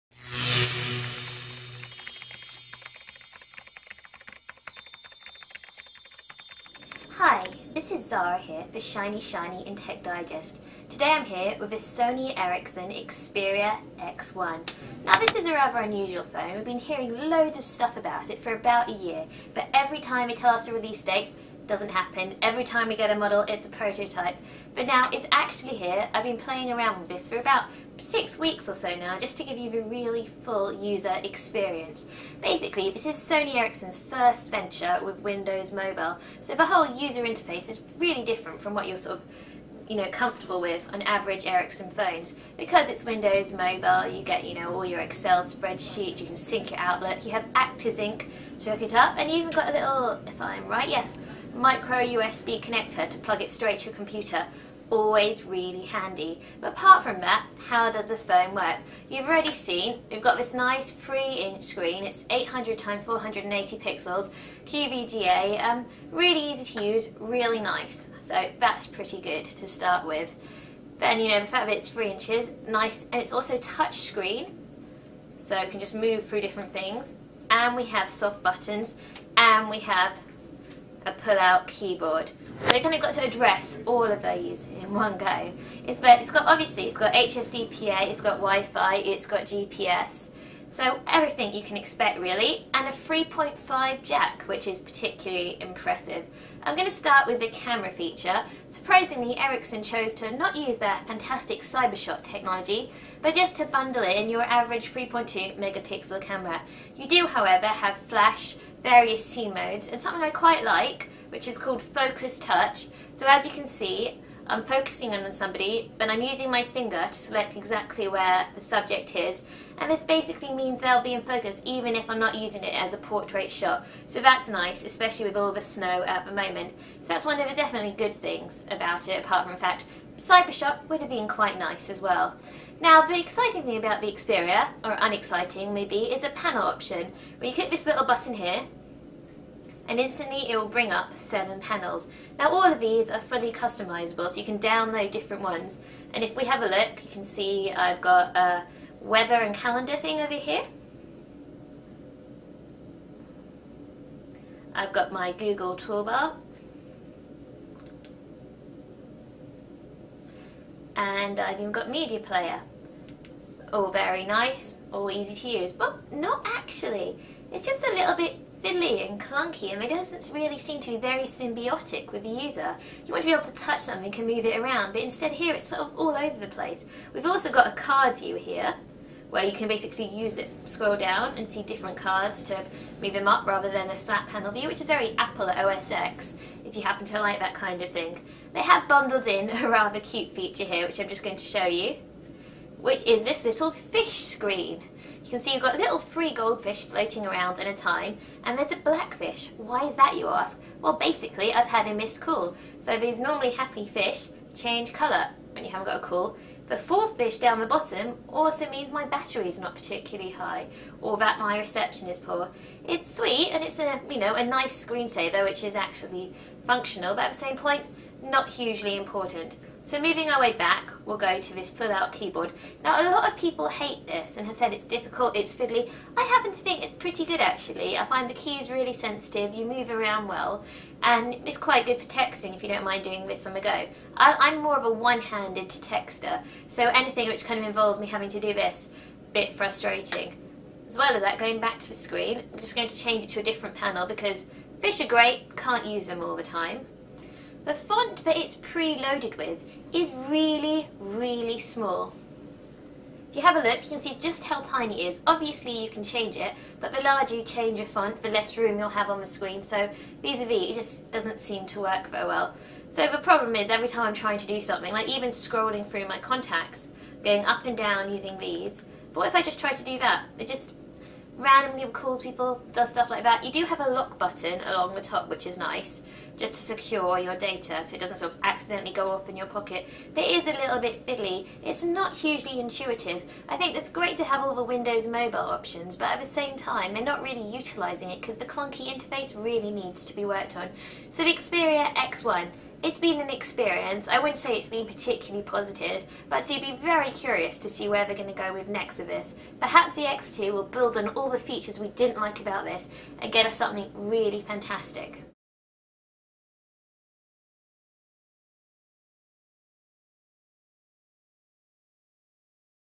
Shiny Review: Sony Ericsson XPERIA X1
Zara looks at the Sony Ericsson XPERIA X1